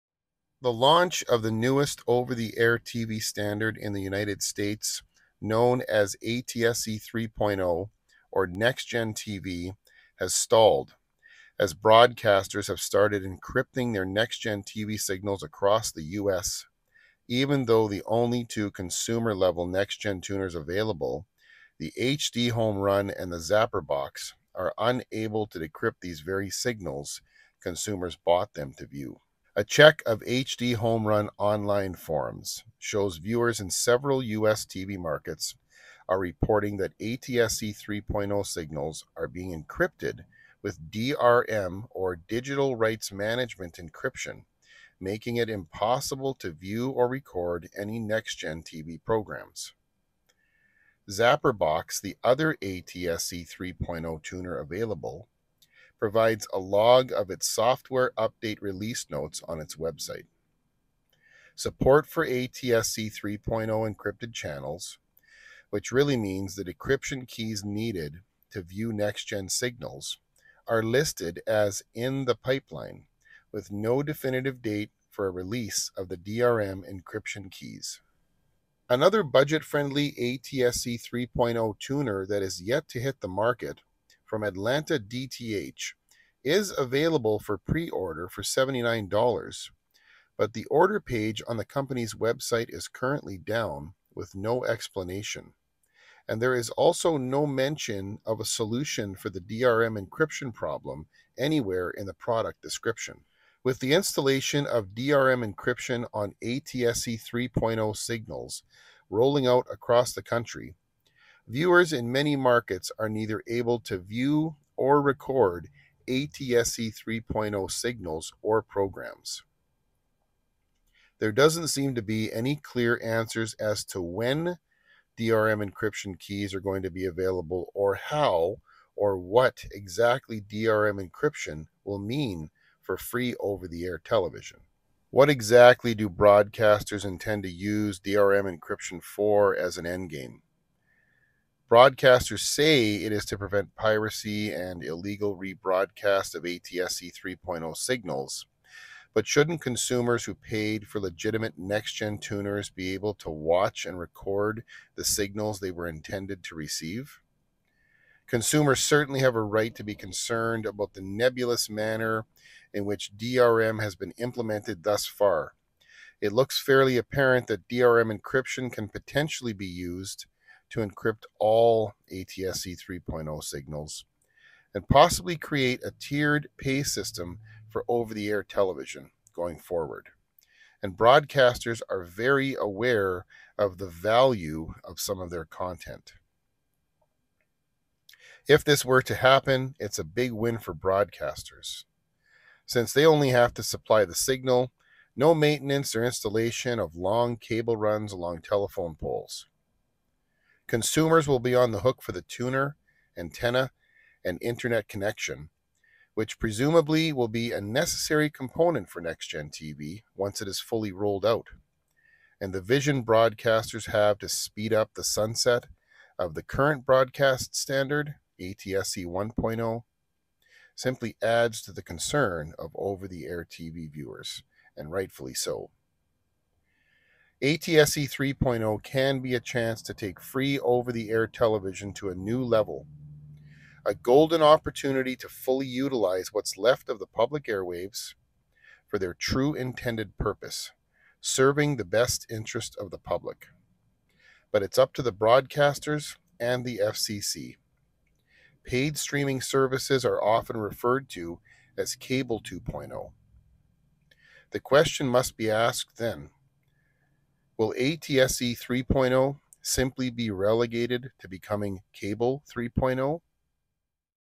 ATSC 3.0 tuners, DRM Encryption, and the future of NextGen TV
ATSC 3.0 tuners, DRM Encryption, and the future of NextGen over the air TV - OTA television
The launch of the newest over-the-air TV standard, known as ATSC 3.0 or Nextgen TV, has stalled, as broadcasters have started encrypting their Nextgen TV signals across the US, even though the only two consumer-level Nextgen tuners available, the HDhomerun and the Zapperbox, are unable to decrypt these very signals consumers bought them to view. Will ATSC 3.0 simply become Cable 3.0?

(UPDATE: store now up and running again!)

Turning old Rabbit Ears into an outdoor TV Antenna for Free Over the Air TV Channels